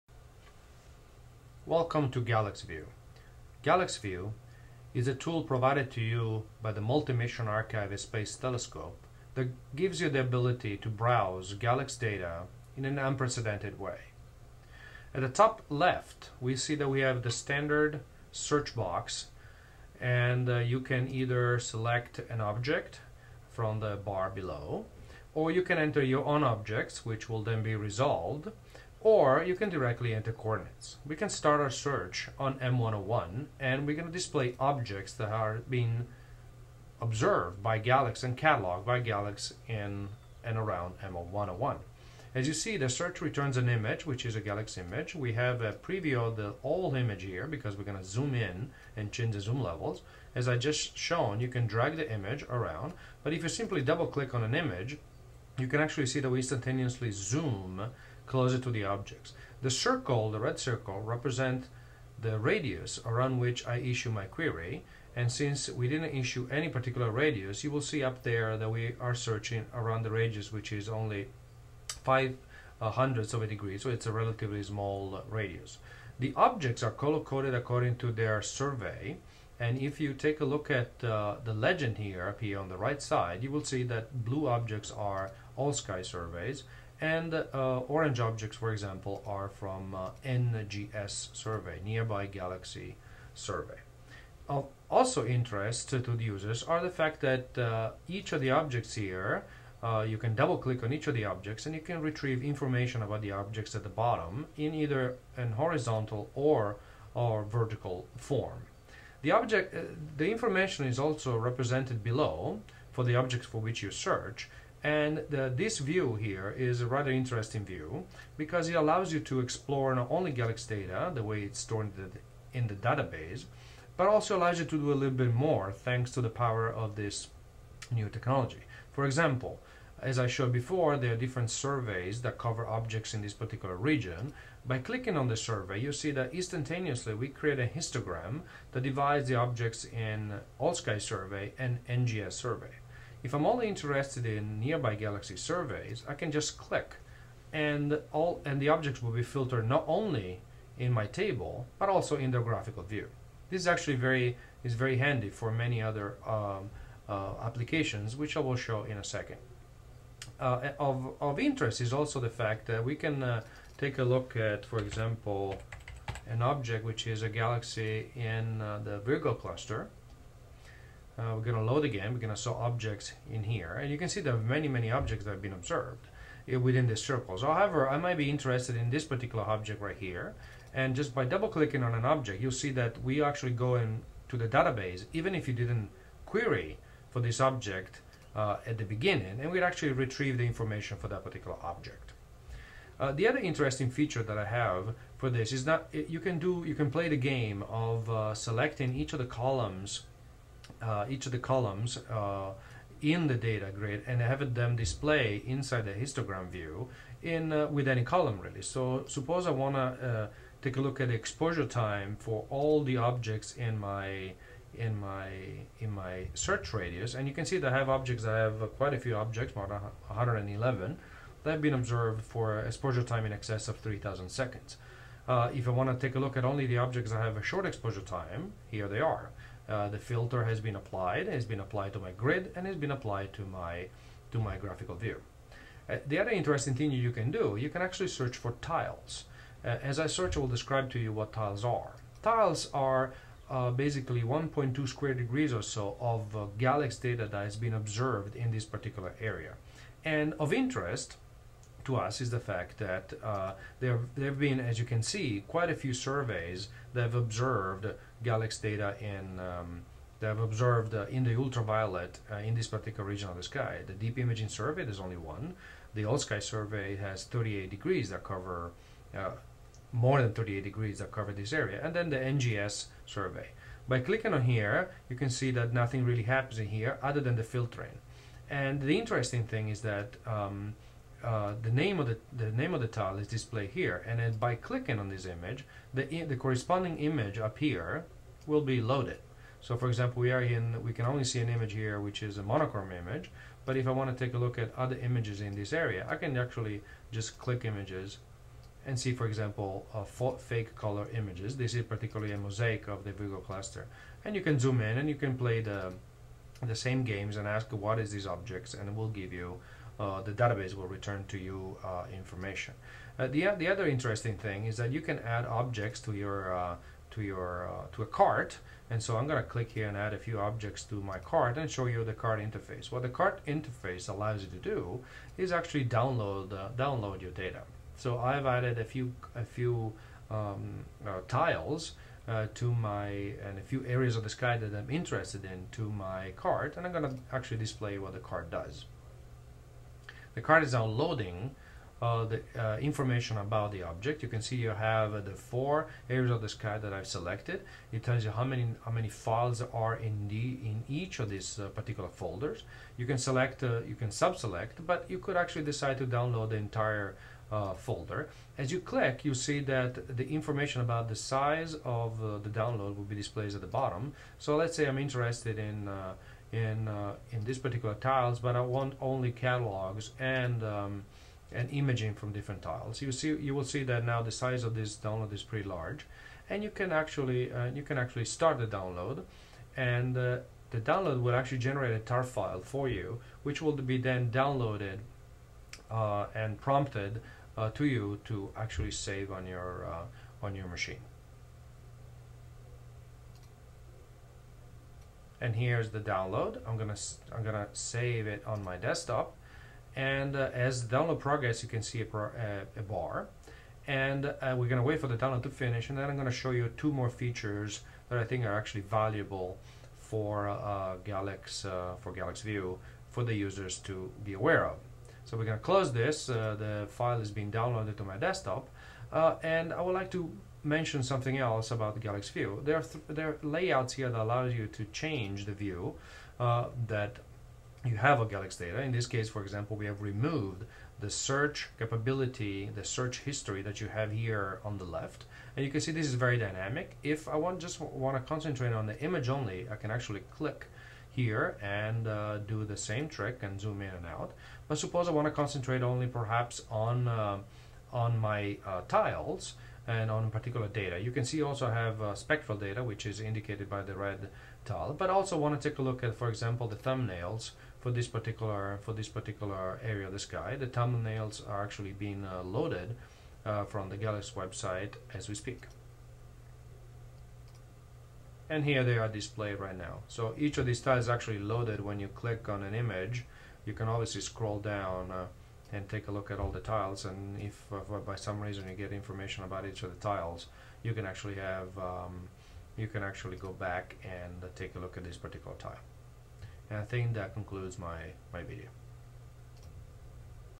Introduction to GalexView
Introduction Tutorial of GalexView
a Flex-based web application for searching, visualizing and downloading astronomical data products from the Galexy Evolution Explorer (GALEX) Public Archive located at MAST.